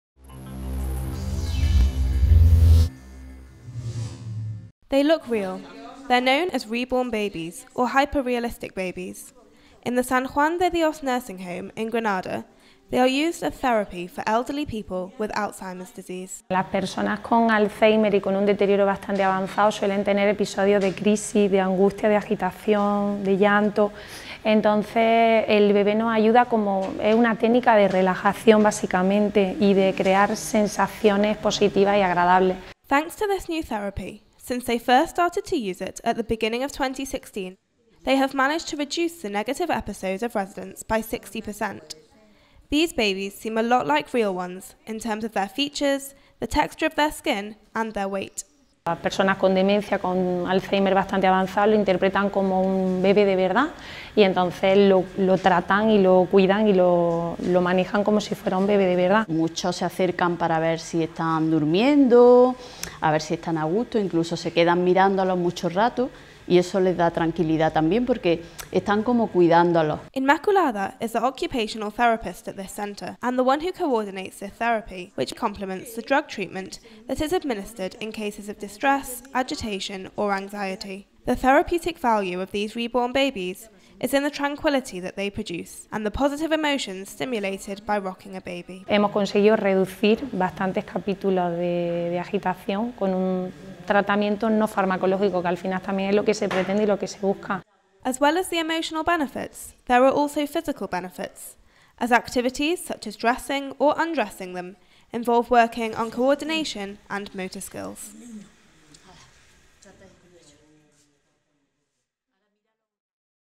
AS Hyper-realistic babies as alternative therapy for elderly people with Alzheimer’s disease
In the nursing home San Juan de Dios in Granada, they use ‘Reborn babies’ as a new therapy to counter the cognitive decline of elderly people with Alzheimer’s disease. They provide relief in moments of agitation or anxiety, and are an alternative to drug treatment. In one year, they have managed to reduce ‘negative episodes’ by 60%.